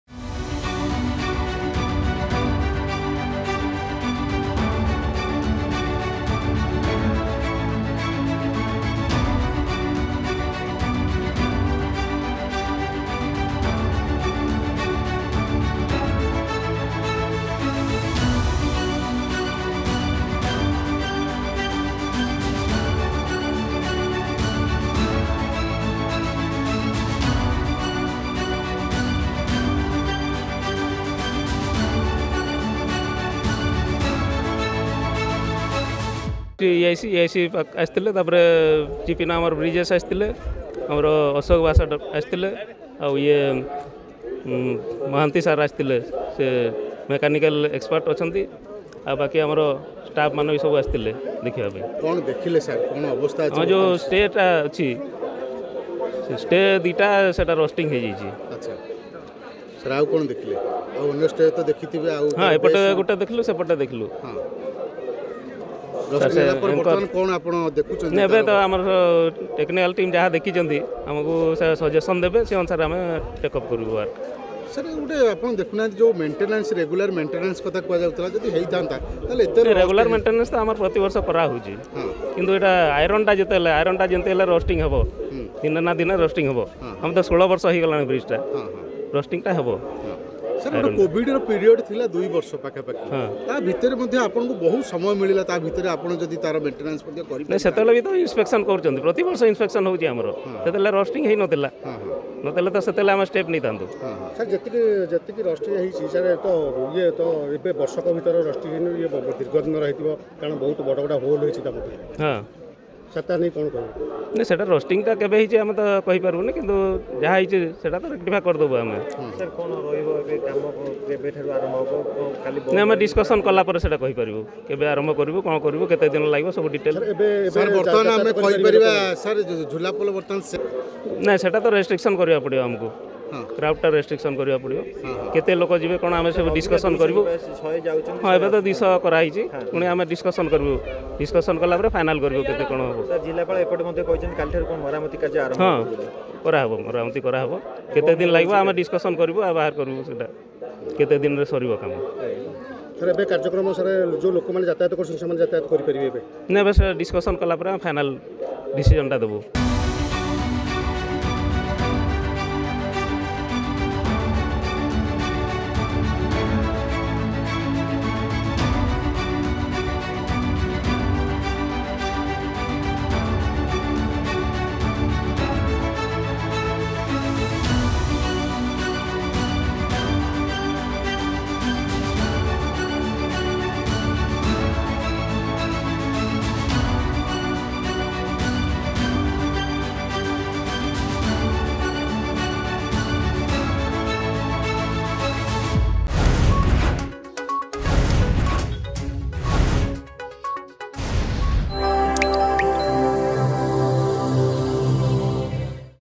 ଝୁଲାପୋଲର ସ୍ଥିତି ଅନୁଧ୍ୟାନ ପାଇଁ ଧବଳେଶ୍ଵରରେ ପହଞ୍ଚିଲେ ବିଶେଷଜ୍ଞ ଟିମ୍
ଝୁଲାପୋଲର ସ୍ଥିତି ଅନୁଧ୍ୟାନ ପାଇଁ ଧବଳେଶ୍ଵରରେ ପହଞ୍ଚିଲେ ବିଶେଷଜ୍ଞ ଟିମ୍ଦୁ
ର୍ବଳ ହୋଇଥିବା ସ୍ଥାନ ବୁଲି ଦେଖୁଛନ୍ତି । ଟିମ୍ ରେ ଅଛନ୍ତି ୮ଜଣ ସଦସ୍ୟ ।